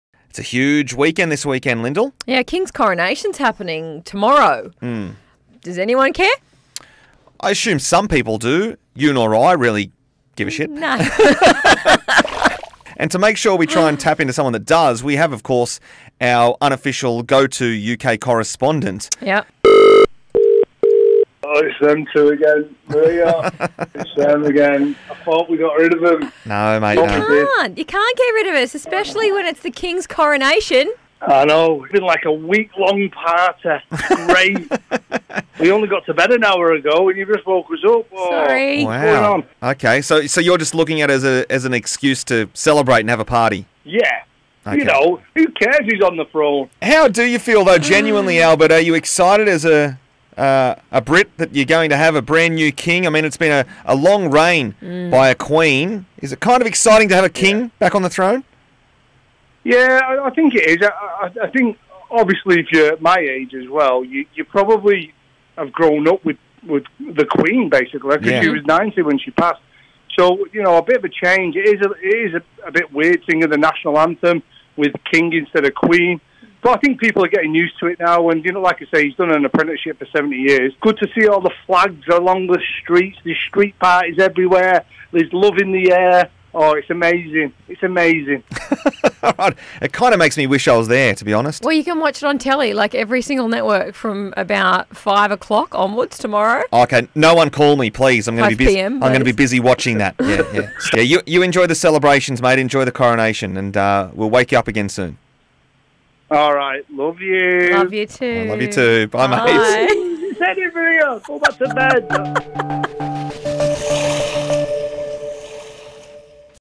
The Kings Coronation
Lyndal and Crammy cross to their UK correspondent to see how the people are reacting to the hype of this weekend's event.